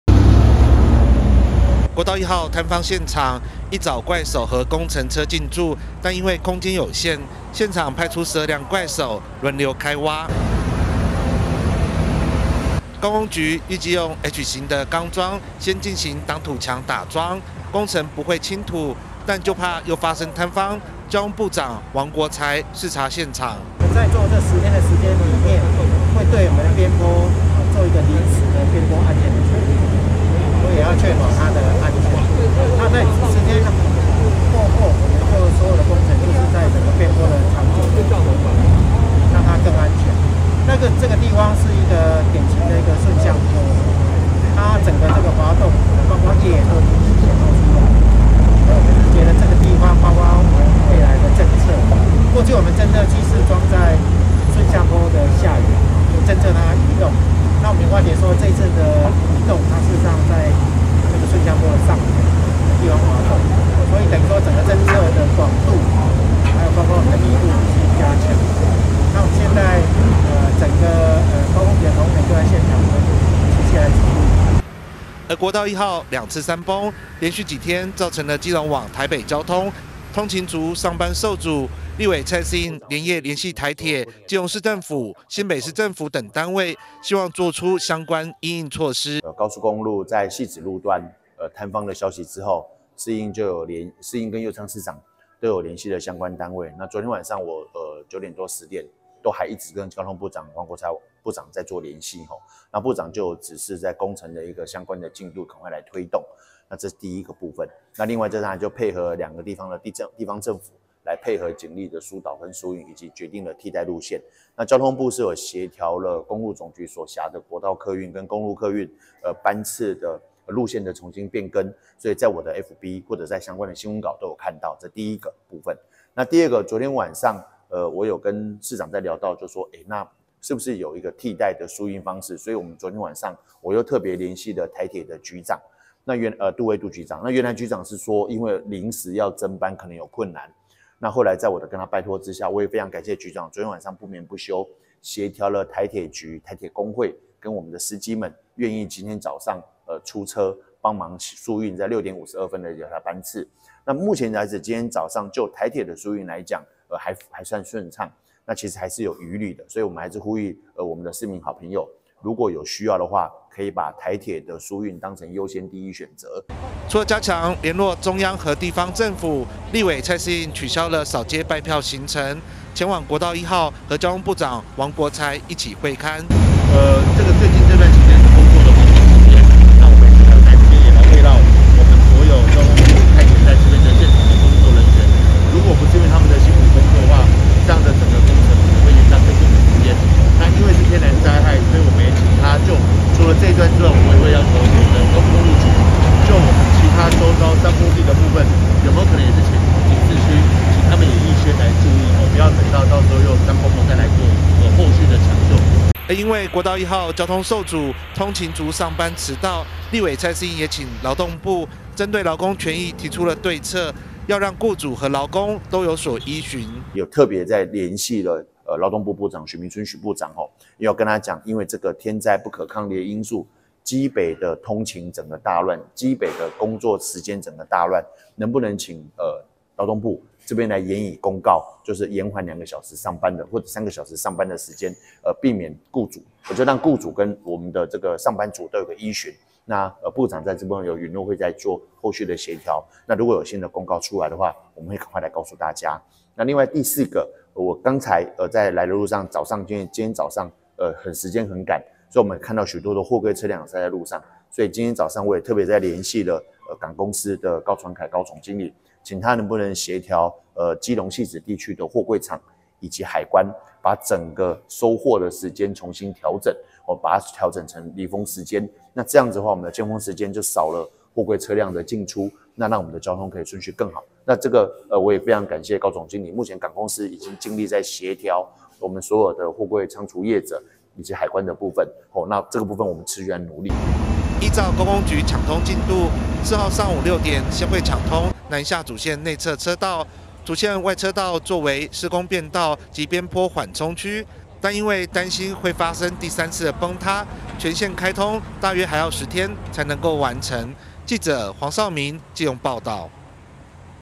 國1走山｜ 國1汐止邊坡走山 蔡適應、王國材會勘現場
國1走山｜ 國1汐止邊坡走山 蔡適應、王國材會勘搶災

(中嘉新聞記者黃紹銘/基隆採訪報導) 國道1號邊坡滑落，基隆通勤交通受阻，上班遲到，叫苦連天，立委蔡適應緊急連絡台鐵、港務局、市府及勞動部，提出因應方案，也緊急取消掃街拜票行程，陪同交通部長王國材到國道一號勘災，交通部長說，希望四日上午6時內側車道可以先行通車，外側車道將做為施工便道及緩衝區，整個開通預估要10天。